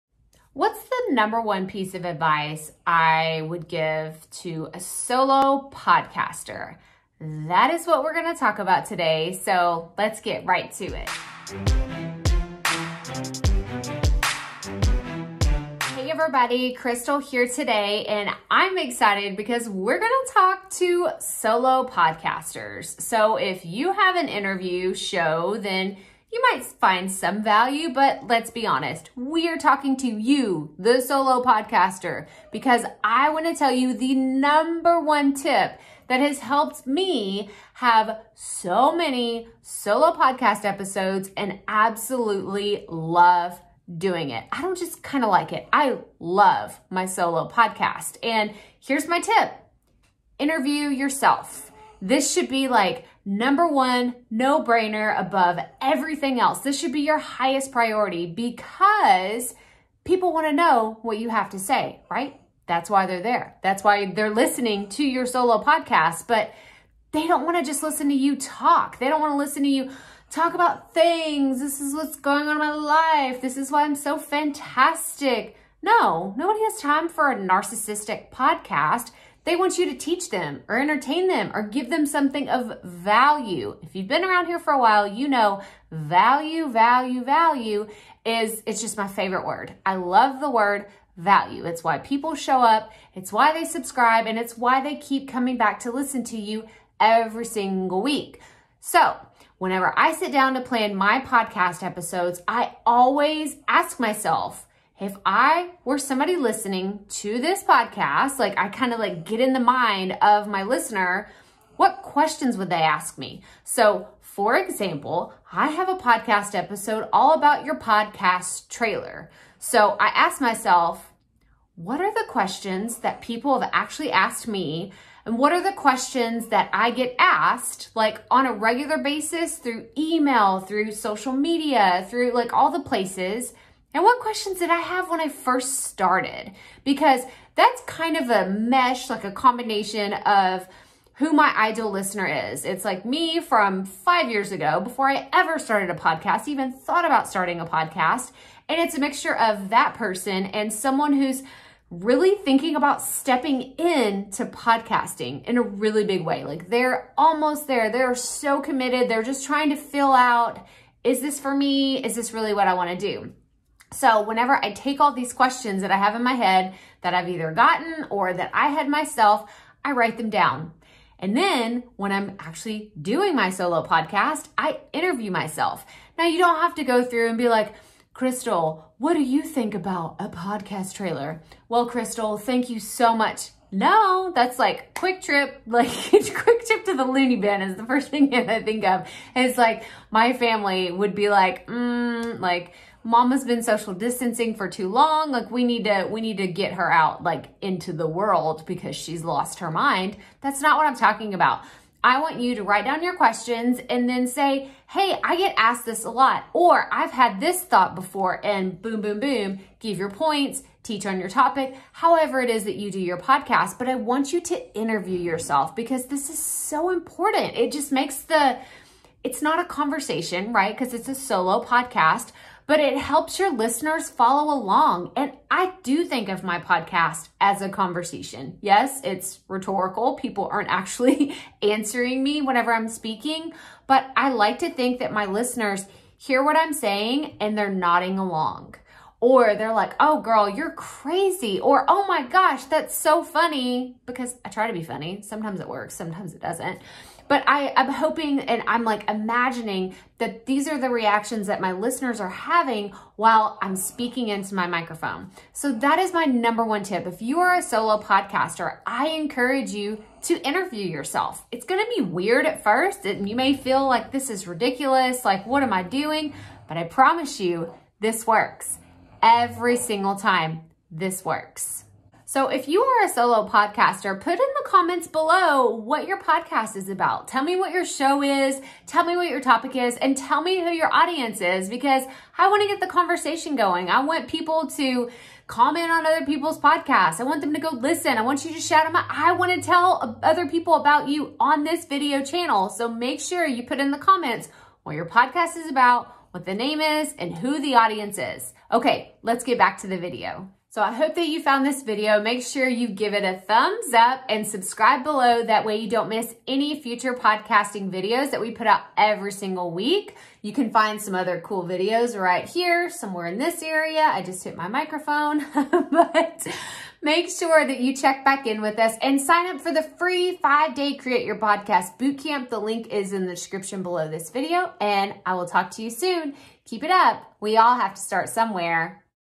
Solo Podcasts, The Number One Tip to Know!
With more than 150+ podcast episodes under my belt, I've found that there's one tip that I always follow for my solo podcasts. Think of it like the "secret sauce" not only having solo podcast episodes but loving every second of recording your solo podcast episodes!

In this video, I'm showing you the number one tip I'd recommend for anyone creating solo podcast episodes. So, let's dive right in!

Check Out the FREE Podcast Training:
"From Struggling to Award-Winning: 4 Insider Secrets Every Podcaster Needs to Know"

Podcast Mentioned:
"Podcast Trailer, What Needs to Be In It?"